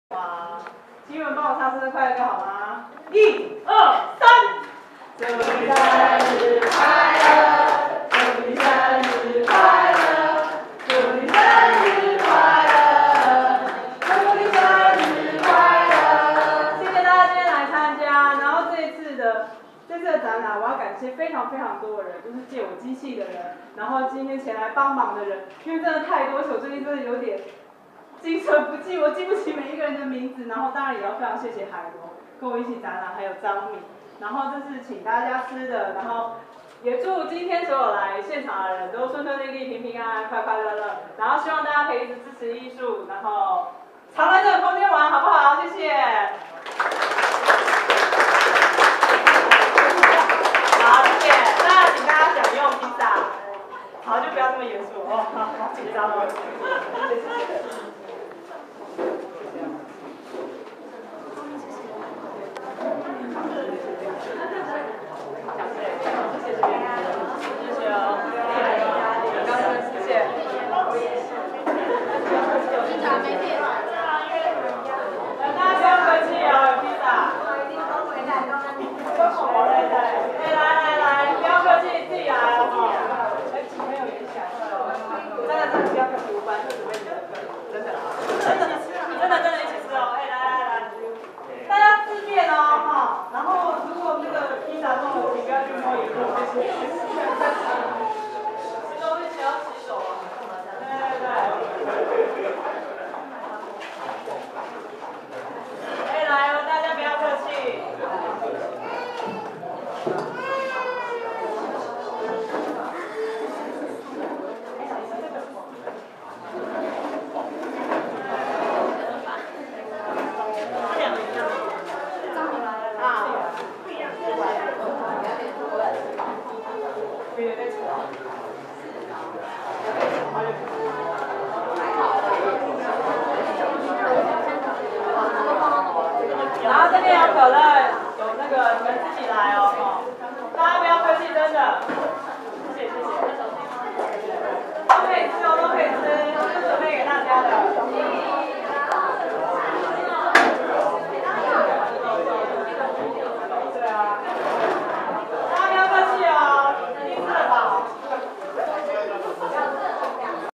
2010一歲生日座談活動（蔡玨伶,蔡珏伶,Chueh-Lin Tsai)
一歲生日最後的活動是切蛋糕,但是我是切pizza,希望每年都能過一次生日,也歡迎朋友一直陪我和我的作品一起長大!!